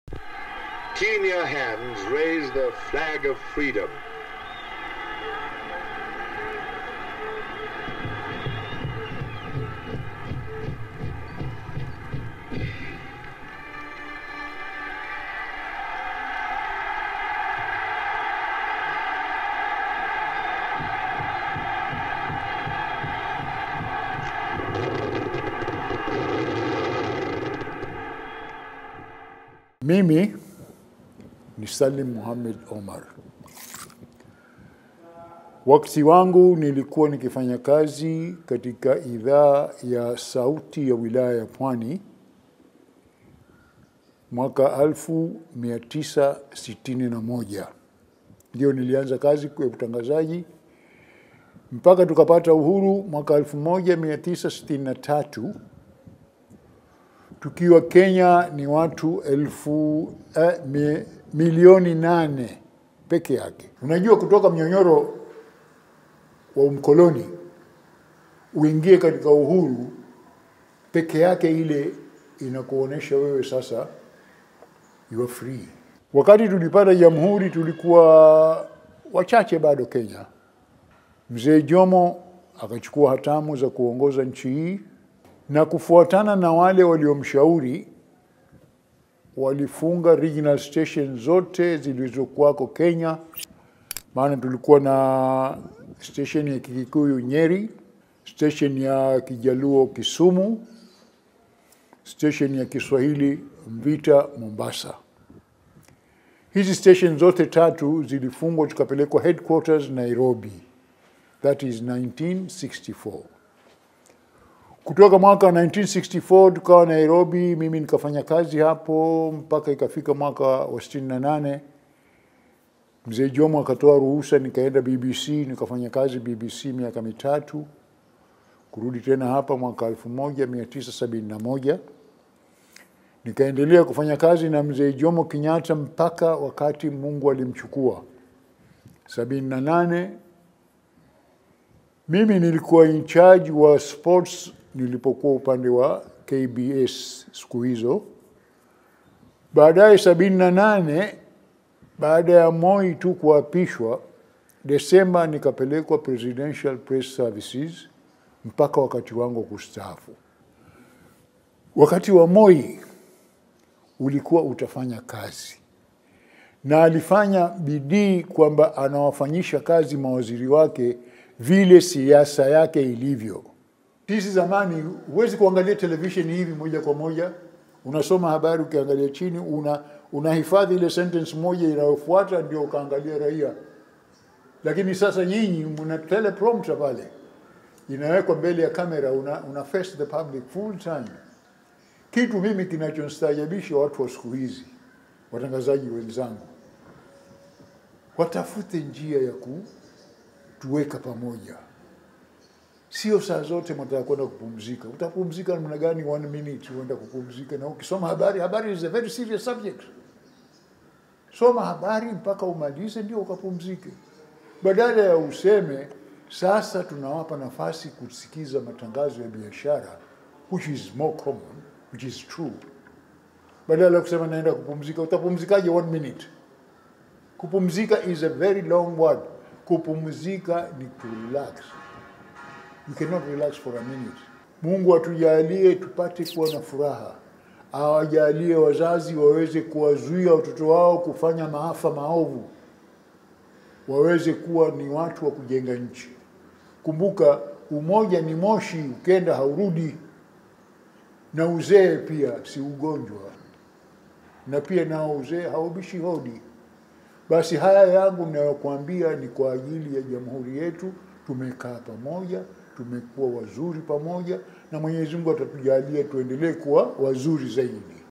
Interview with Salim Mohamed Omar -veteran Presidential Press Service broadcaster || 11th Dec 2020
Profiling our journey as a republic thus far in the context of media personalities, we have produced broadcasters of note—among them Salim Mohammed Omar who served under President Jomo Kenyatta and President Daniel Moi in the Presidential Press Service.